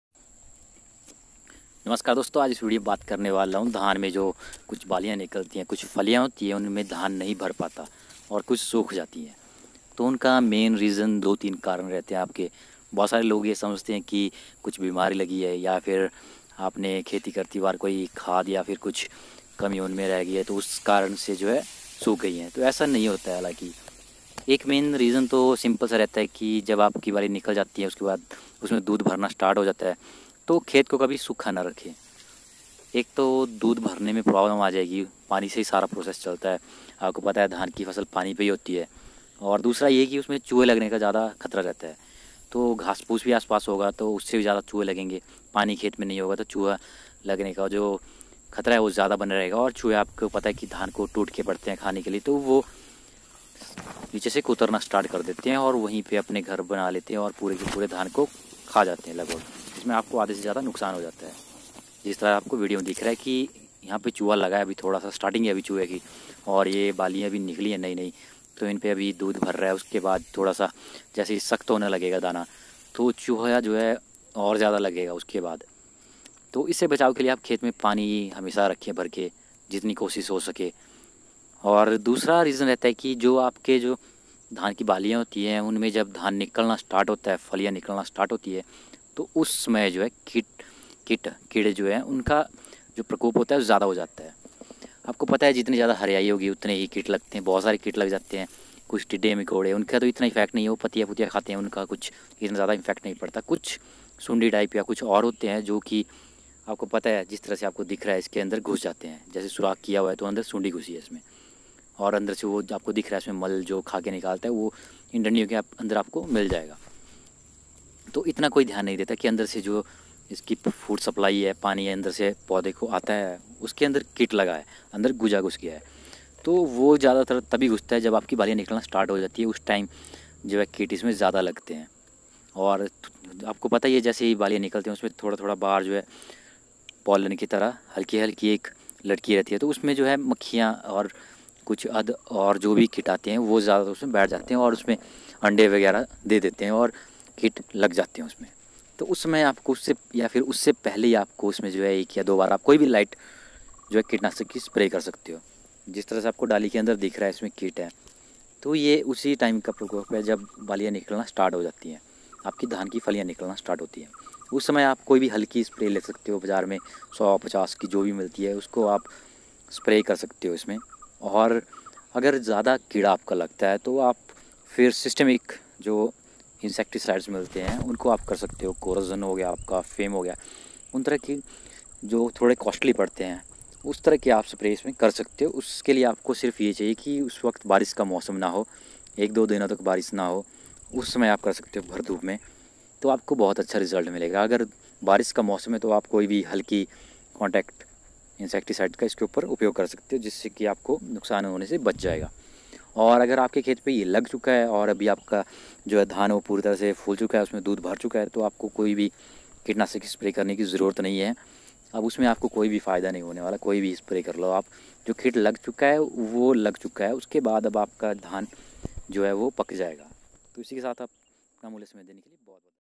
Dhan me lgane wala keede | धान की खेती | rice farming | paddy insecticide | indian farming
Dhan ki fasal me lagne wale keede jin se dhaan sukh jata h

Aggar koi swal ya sujhav reh jata h to comment box me jrur likhe